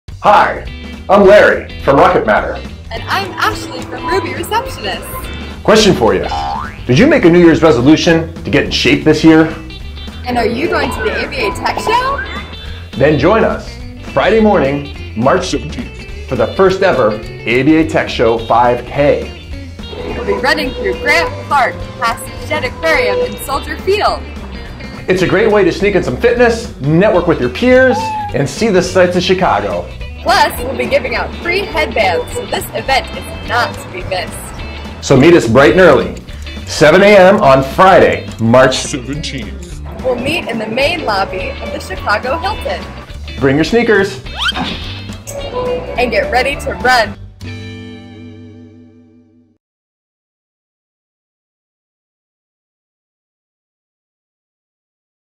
ABA TECHSHOW 5K 2017
Rocket Matter and Ruby Receptionists are proud to bring you the FIRST EVER ABA TECHSHOW 5K! (Registration info in description below)

Join us at 7am in the lobby of the Chicago Hilton to run with us! It's a great opportunity to see the sights of Chicago while sneaking in some fitness and networking with your peers!

Bring your friends and colleagues! See you there!

DATE: Friday, March 17th
TIME: 7:00am Central
ADDRESS:
720 SOUTH MICHIGAN AVENUE